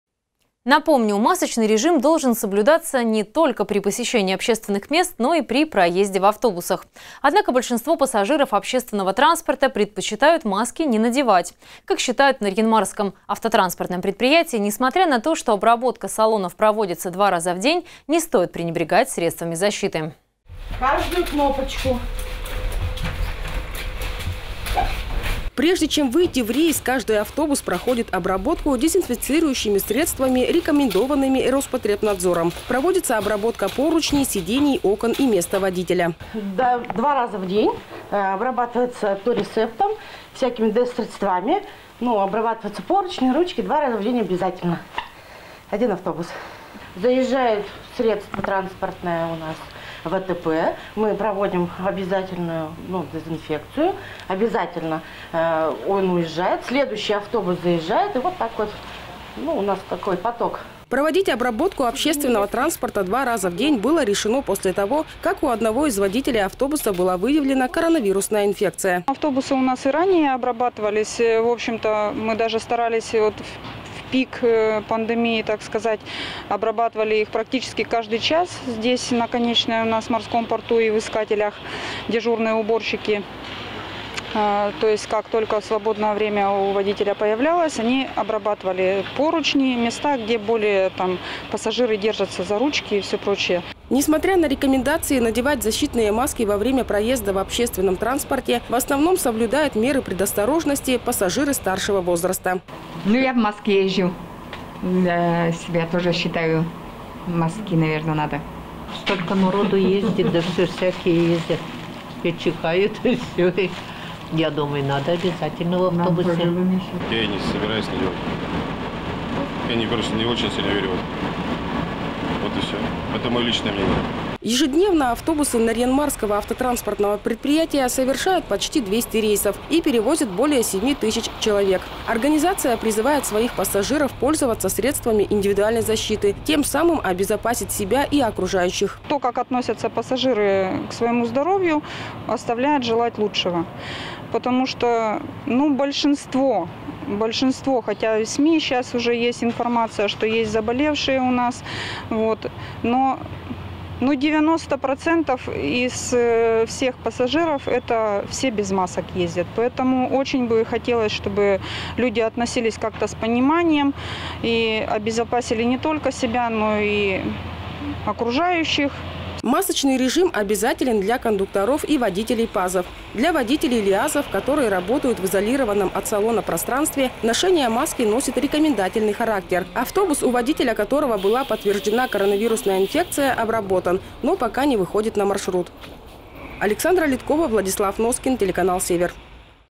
В ЦЕЛЯХ ПРОФИЛАКТИКИ
Напомним, масочный режим должен соблюдаться не только при посещении общественных мест, но и при проезде в автобусах. Однако, большинство пассажиров общественного транспорта предпочитают маски не надевать. Как считают в Нарьян-Марском автотранспортном предприятии, несмотря на то, что обработка салонов проводится два раза в день, не стоит пренебрегать средствами защиты.